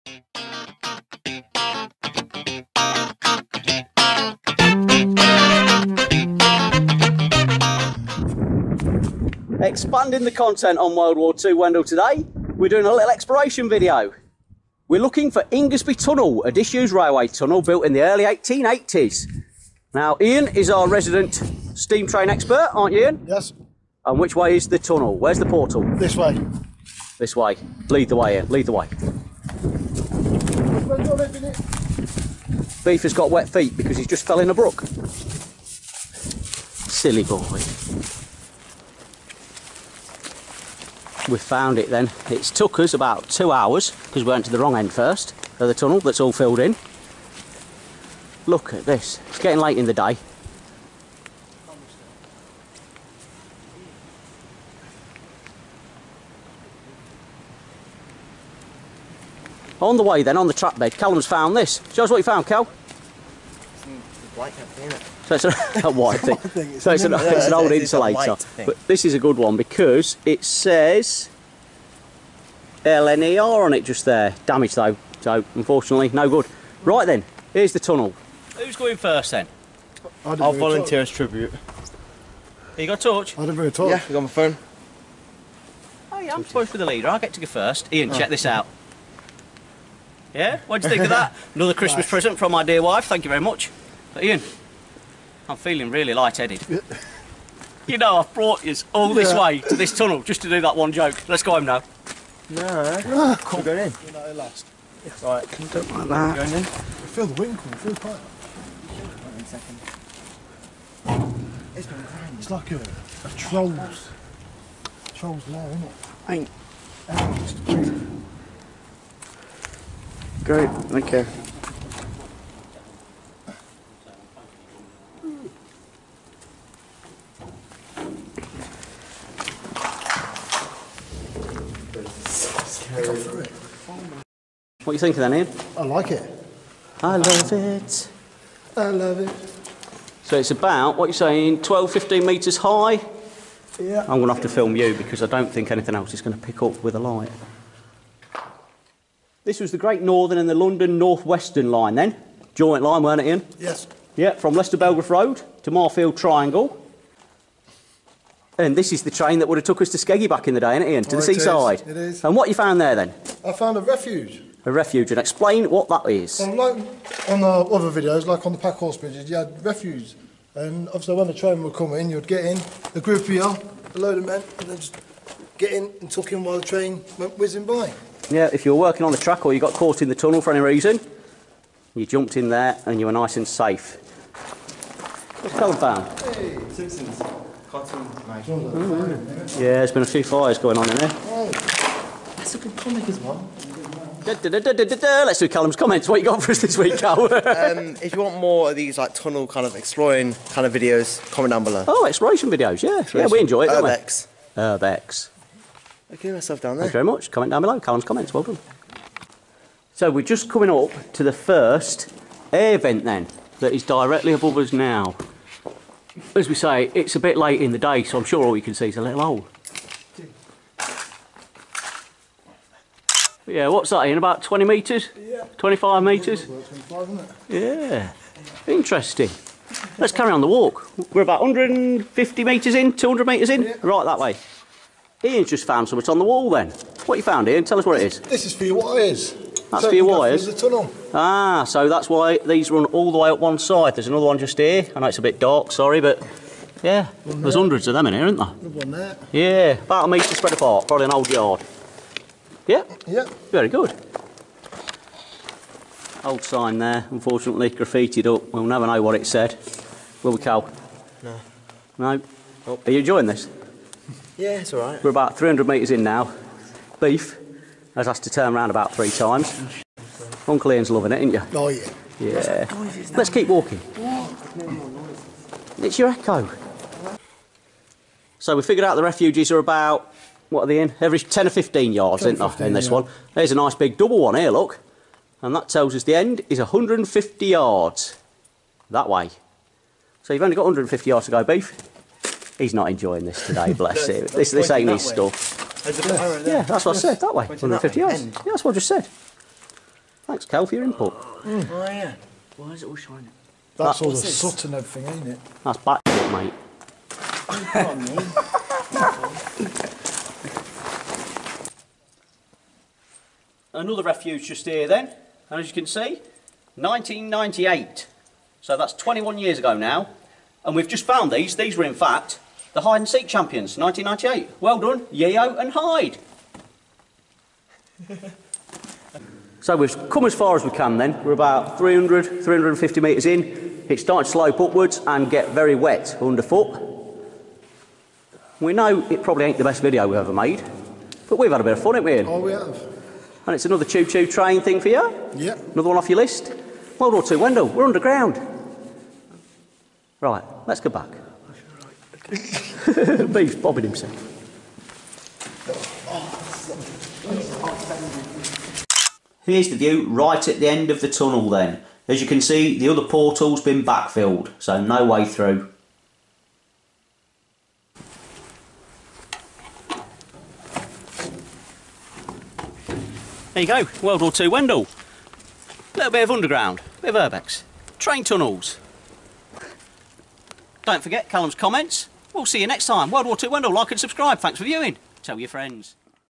Exploring abandoned rail tunnel at night!
The Wendal boys go underground in this episode.

We explore the long abandoned and once blocked Ingarsby tunnel in Leicestershire

The tunnel opened as part of the Great Northern Railway Leicester branch in 1882 and closed 1957 it's length when opened stretched to 516 yards (472 metres).

By the time we found the entrance night had fallen leading to an explore in almost total darkness.

New videos uploaded every Tuesday and Friday 7pm UK time.